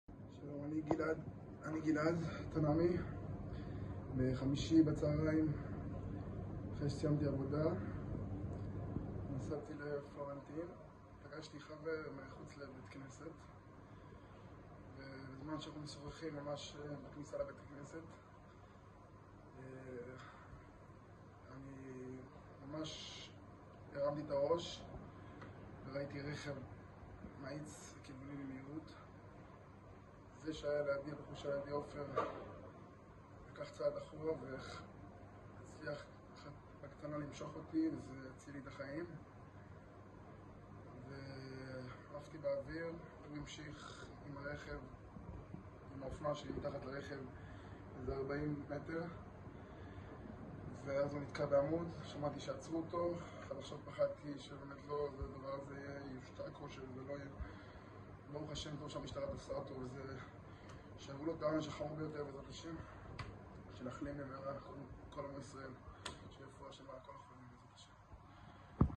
המשטרה והשב"כ חוקרים: ייתכן והדריסה ביום חמישי - פיגוע
"ידעתי מהתחלה": במשטרה חושדים כי התאונה ביום חמישי - פיגוע
ביום חמישי האחרון נדרס רוכב אופנוע ברחוב אברבנאל בתל אביב. במשטרה ובשב"כ חושדים עתה כי הדריסה הייתה מכוונת וכי מדובר בפיגוע טרור